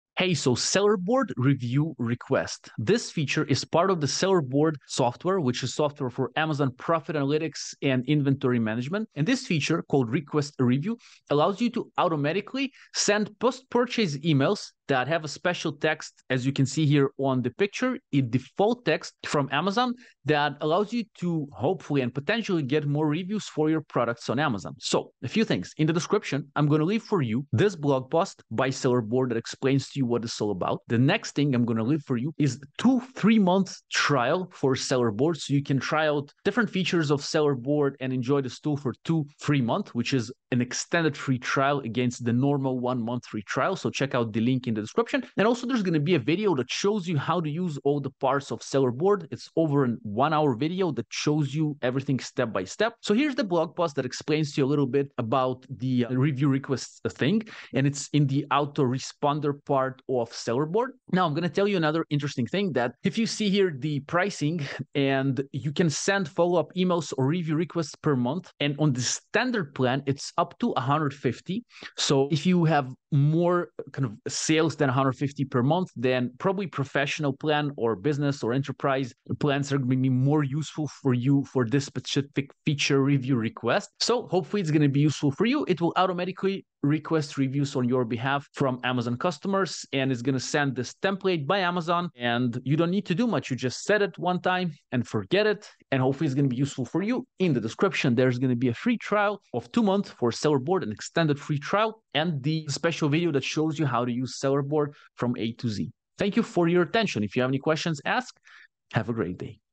Sellerboard Review Request - Automation Tool And Software For Bulk Review Requests Email On Amazon
Get more Amazon reviews on autopilot with the Sellerboard review request feature. It's an awesome software and tool for bulk review requests. Instead of pressing the request a review button yourself, install the tool and automate it once and for all. Get it below. Enjoy!

📌 Useful Links From The Video 📌

Getida:
(free account with lower rates)

📌 Hashtags 📌